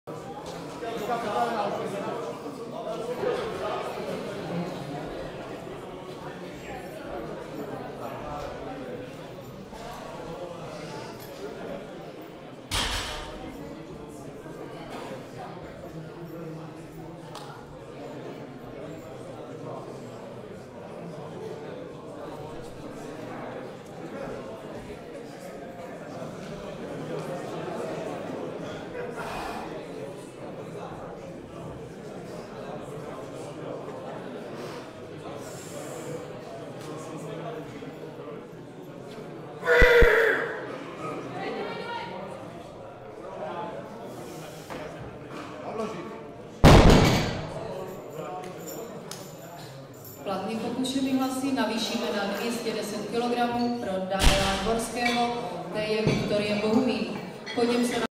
Goliáš Pavel (1998), MT2, 197,5 kg
1. Mistrovství Moravy v klasickém (RAW) silovém trojboji dorostu a juniorů

Místo: Hornický dům, Zbýšov